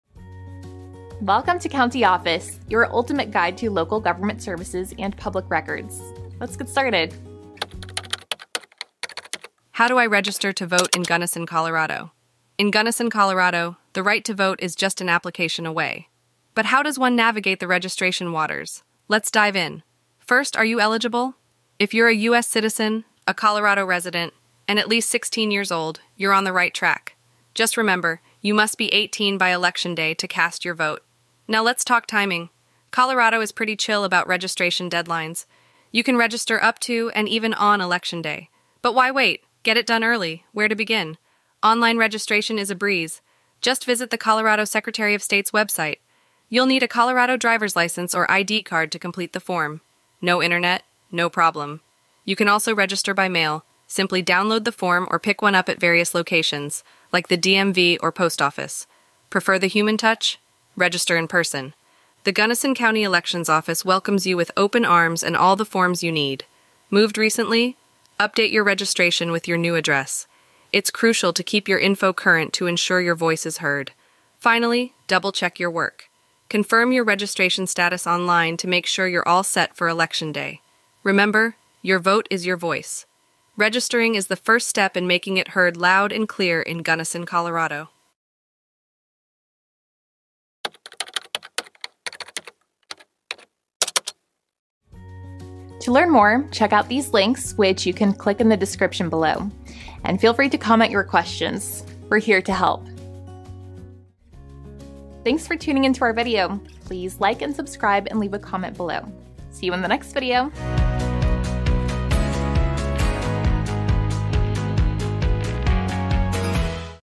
How Do I Register to Vote In Gunnison County, Colorado? - CountyOffice.org
How Do I Register to Vote In Gunnison County, Colorado? Navigating the voter registration process in Gunnison, Colorado, is essential for ensuring your voice is heard in upcoming elections. This video provides a comprehensive guide to getting registered so you can participate in our democracy. Whether you prefer the convenience of online registration, the traditional mail-in method, or the personal touch of registering in person, we've got you covered with all the details you need. Plus, we'll share tips on keeping your registration up to date, especially if you've recently moved. Don't leave your voter registration to the last minute; follow our step-by-step instructions and get ready to make an impact on Election Day.